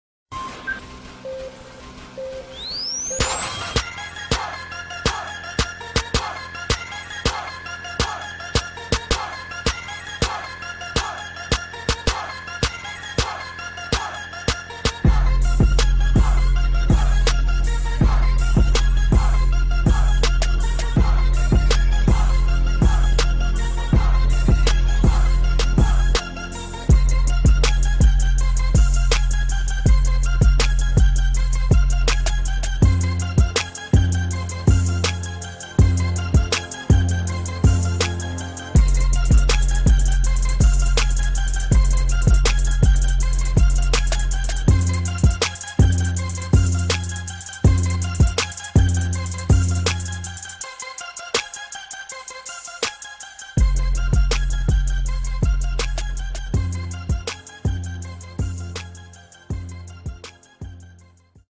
James Bolden Highlights v Moeller
Bolden finished with 26 points in this scrimmage against Moeller. These are just some of the highlights.